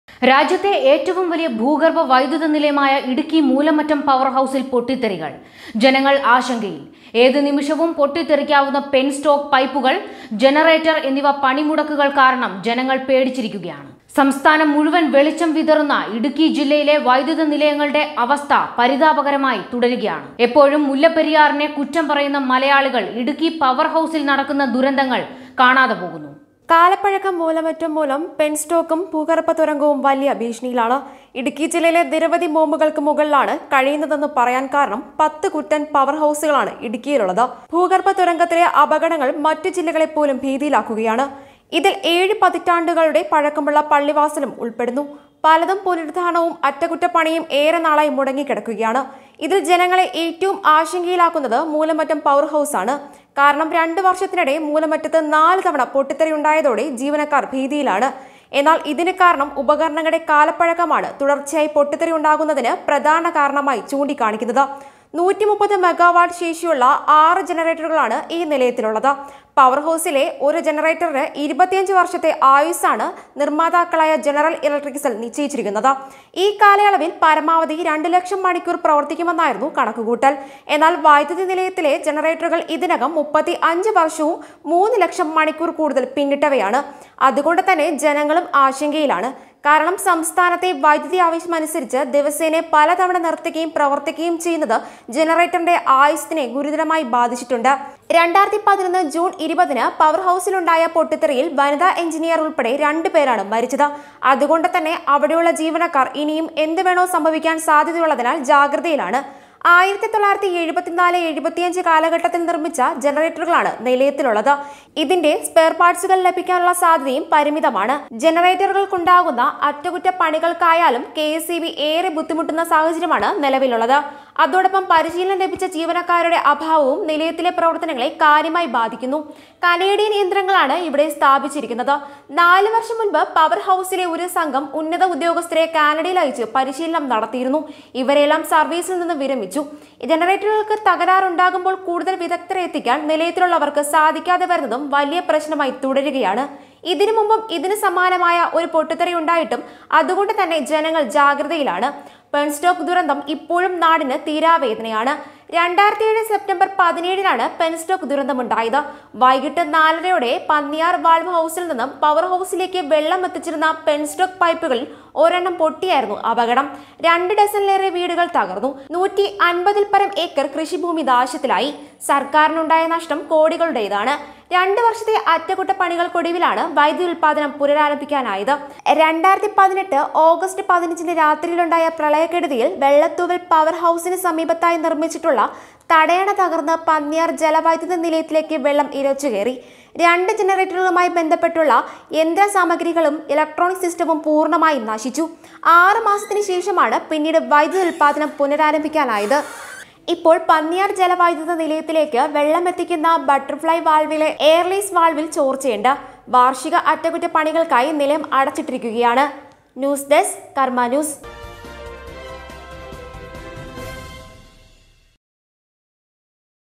ഇടുക്കി വൈദ്യുതി ബോംബിനു മുകളിൽ
മുല്ലപെരിയാറിന്റെ പുറകേ പോകുമ്പോൾ ഇടുക്കി പവർ ഹൗസിലെ ദുരന്തം മലയാളികൾ കാണുന്നില്ല, ഭൂഗർഭ വൈദ്യുതി നിലയത്തിൽ പൊട്ടിതെറി, കാലപഴക്കം മൂലം വൻ ആശങ്ക